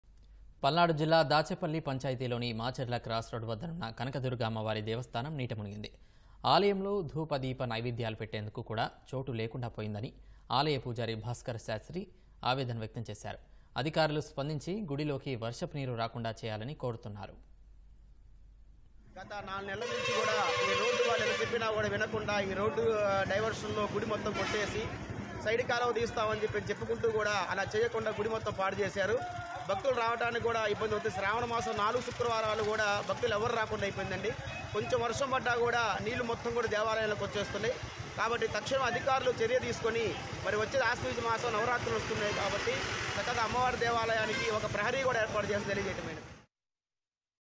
నీటమునిగిన కనకదుర్గ అమ్మవారి దేవస్థానం | Rain Water Enter In Temple | Macherla
పల్నాడు జిల్లా దాచేపల్లి పంచాయతీలోని మాచర్ల క్రాస్  రోడ్  వద్దనున్న కనకదుర్గ అమ్మవారి దేవస్థానం నీటమునిగింది. ఆలయంలో ధూప, దీప నైవేద్యాలు పెట్టేందుకు కూడా చోటు లేకుండాపోయిందని... ఆలయ పూజారి భాస్కర్ శాస్త్రి ఆవేదన వ్యక్తం చేశారు. అధికారులు స్పందించి గుడిలోకి వర్షపు నీరు రాకుండా చేయాలని కోరుతున్నారు